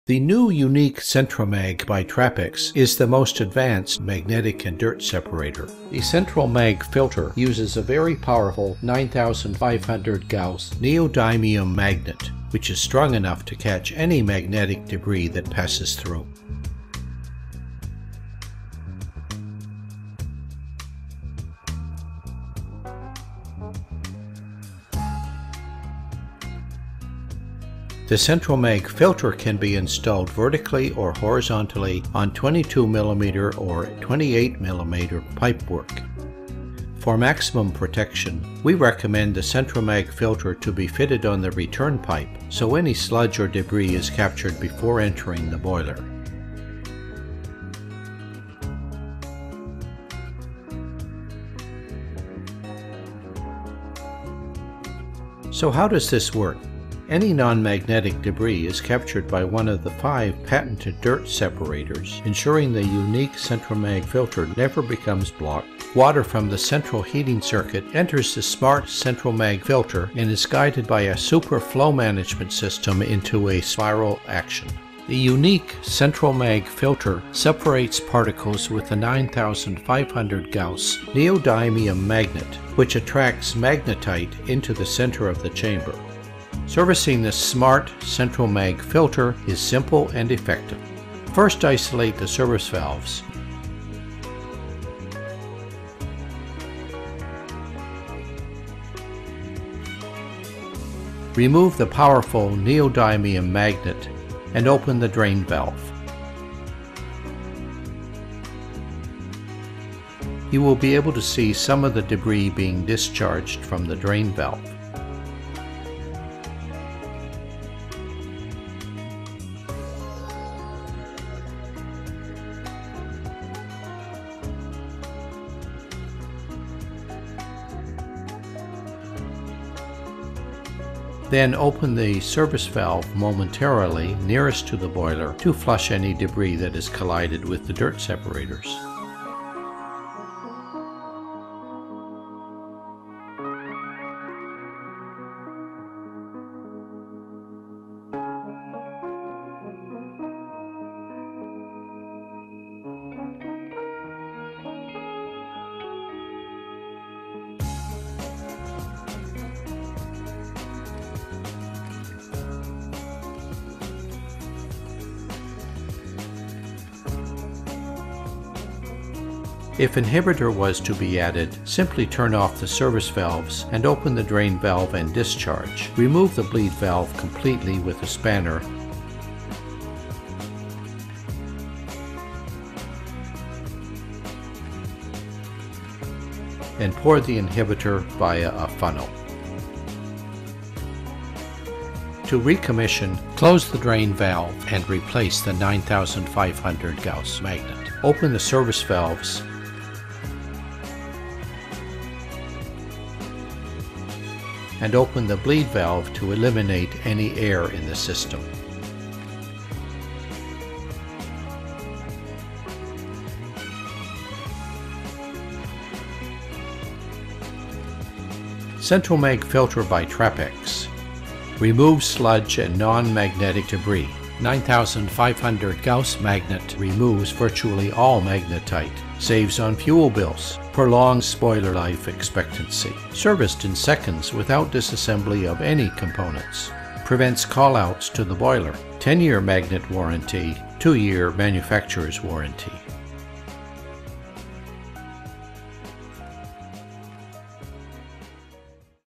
Trappex - Advanced magnetic and non-magnetic particle filter
Centramag has gone one step further as a dual action central heating filter, capturing non-magnetic deposits with the help of the powerful 9500 gauss neodymuim magnet. Centramag has a unique spiral action, forcing the flow through 5 dirt separators.